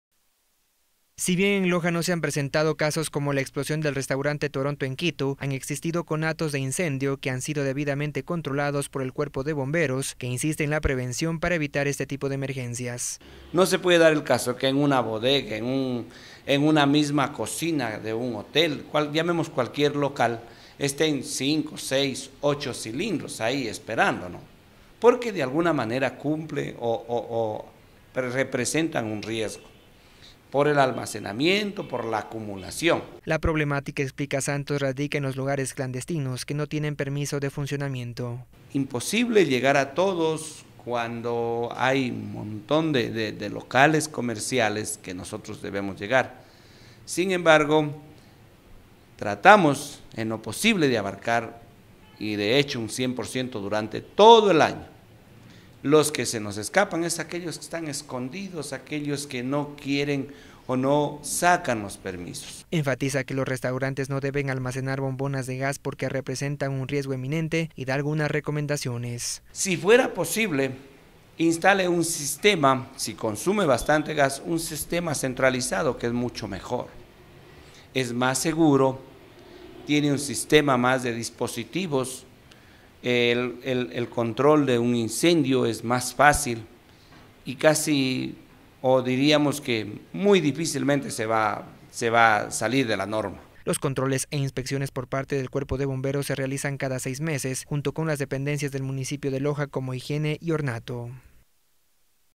Controles para la prevención de incendios en locales comerciales
Si bien en Loja no se han presentado casos como explosiones en restaurantes, han existido conatos incendios que han sido controlados por el cuerpo de Bomberos que insiste en al prevención para evitar este tipo de emergencias.